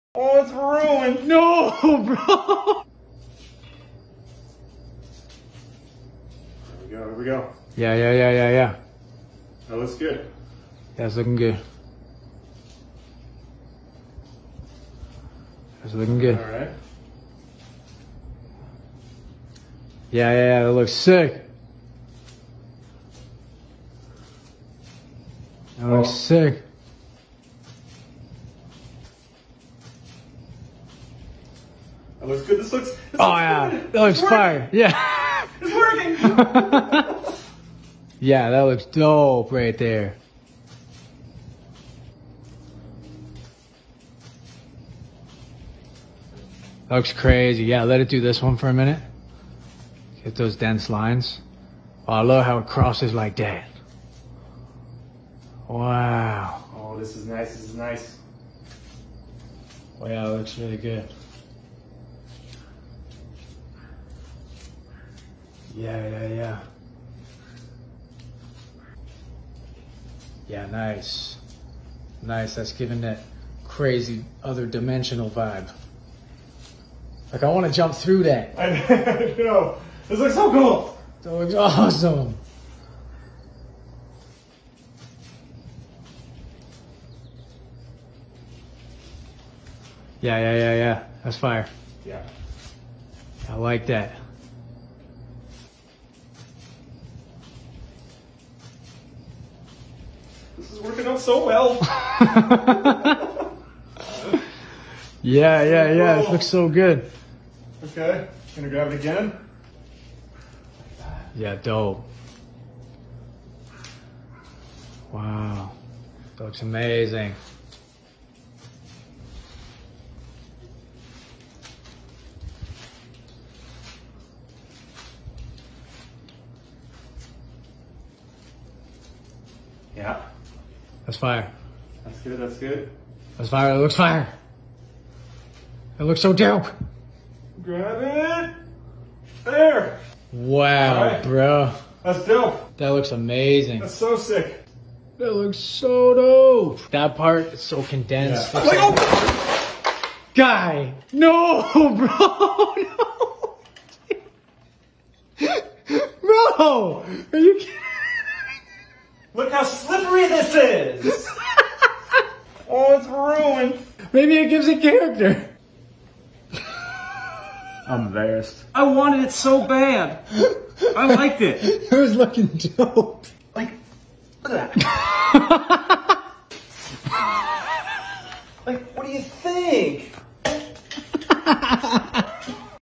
Artist slips & ruins epic masterpiece 😱, It's gone
This is an Epic Video, Artist slips and ruins his wonderful work, see the video and enjoys his work before he slips and destroyed it all.